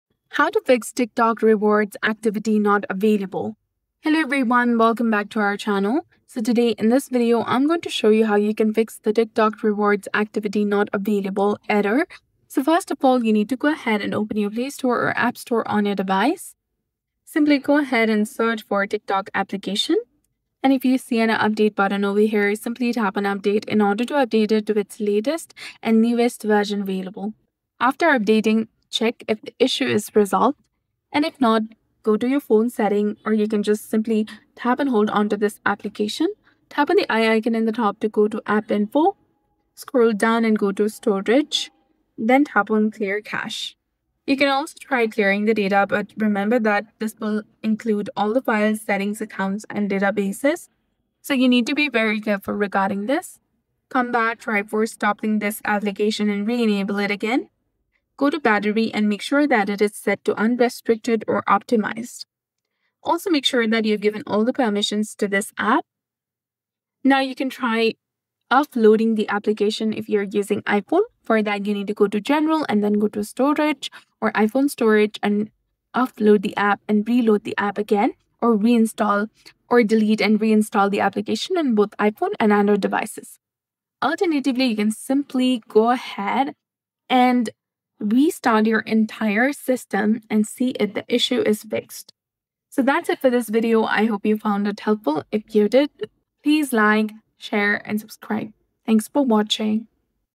How To Fix TikTok Rewards Activity Not Available (2025) TikTok Rewards Not Working Problem Solved
Can’t access the Rewards Activity on TikTok in 2025? This video is your step-by-step guide to troubleshoot the “Rewards Activity Not Available” error, reset your promo features, and get back to earning points and gifts—covering account-eligibility checks, app-update fixes, region-lock workarounds, and quick cache-clear tricks so you can unlock TikTok Rewards again.

Timestamps
0:00 Introduction
0:10 How To Fix TikTok Rewards Activity Not Available